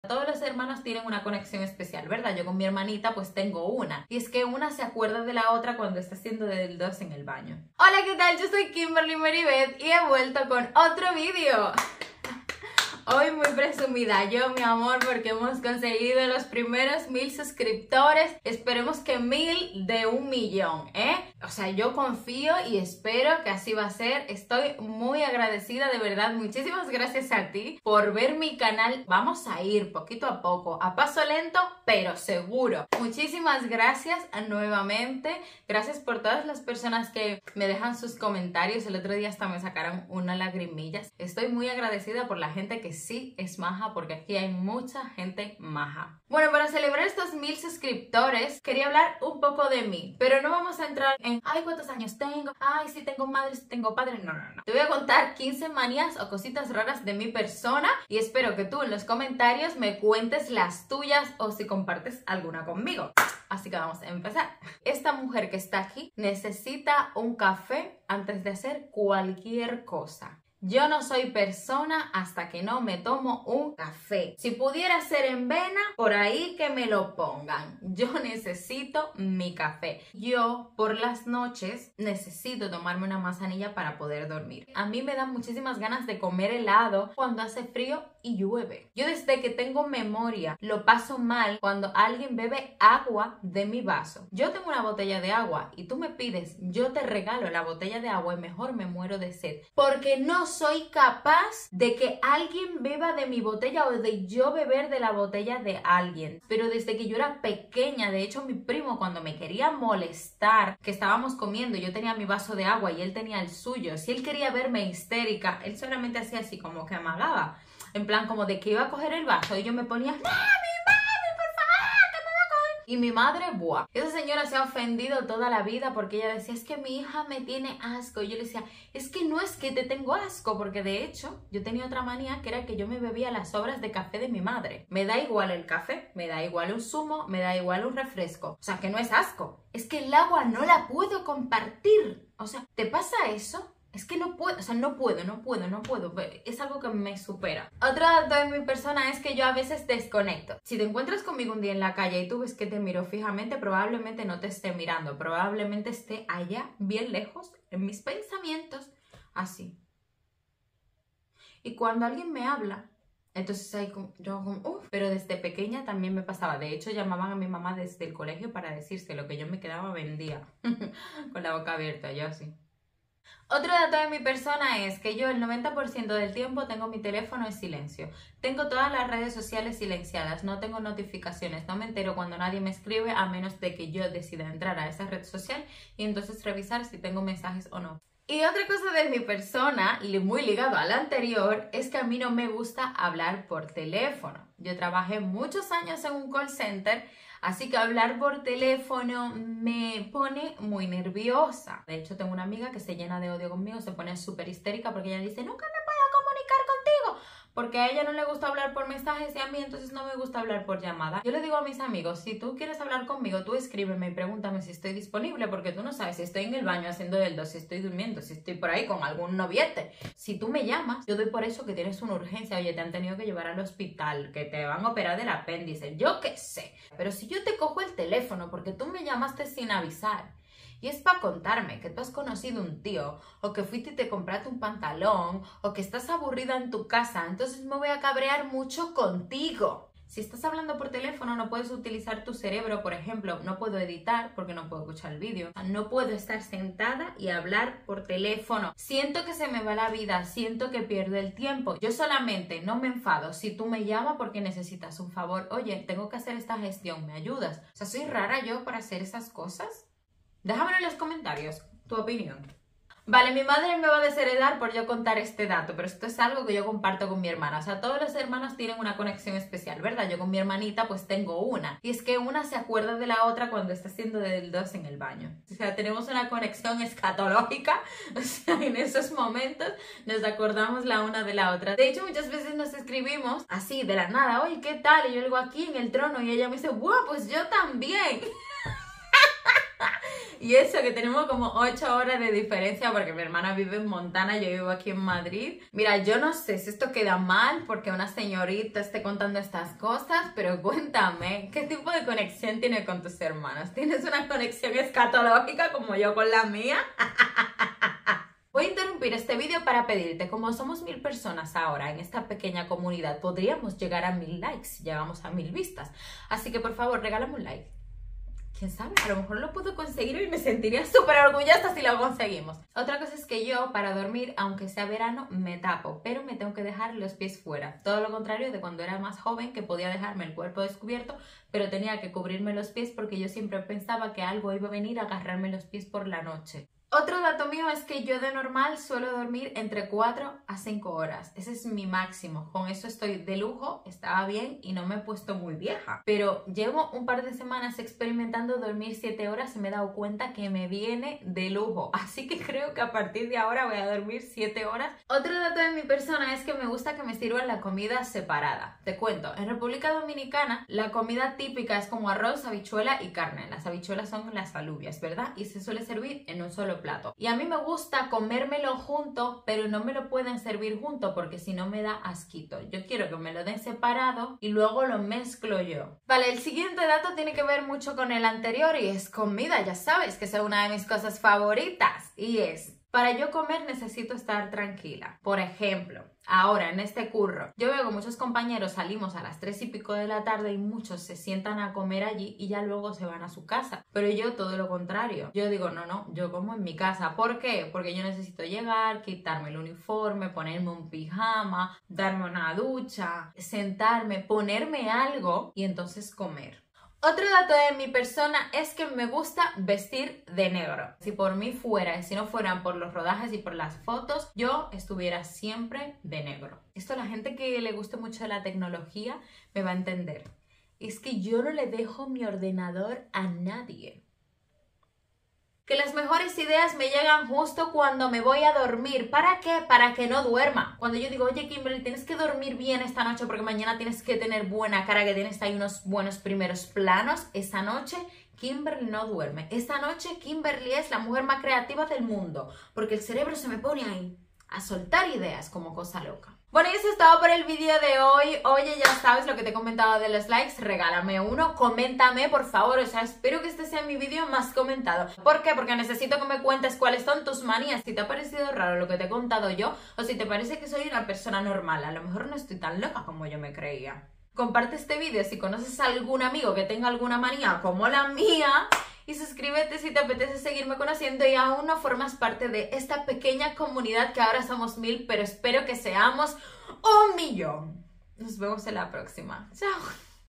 MIS 15 SECRETOS🤫 | Ya SOMOS 1000 😊🎉 vamos a conocernos
En este video, ¡comparto 15 cosas sobre mi, no las típicas sino las mas rarillas pero creo que te podría dar una idea de quien soy. Vamos a echarnos unas risas te va a encantar!

Te cuento todo desde mi amor por el café hasta mi necesidad de dejar un pie fuera de la sábana para dormir, ¡te sorprenderás con las cosas extrañas que me gustan! Además, te cuento cómo mi hermana y yo tenemos una relación escatológica única y cómo las buenas ideas solo llegan justo por la noche para causarme insomnio. ¡Ven a conocerme mejor y diviértete con mis manías en este video!

ÍNDICE
0:00 Introducción
1:03 Manías con: Café, Manzanilla y Helado
1:28 No puedo compartir el agua
2:28 Me quedo desconectada
2:53 Móvil silenciado y no me gusta hablar por teléfono
4:23 Tengo una conexión escatológica con mi hermana
5:08 Quiero llegar a mil likes en este video
5:23 Me tapo aunque haga calor, duermo 4 o 5 horas
5:55 Comida separada, Comer relajada
6:47 Vestir de negro, No presto mi portátil y Las mejores ideas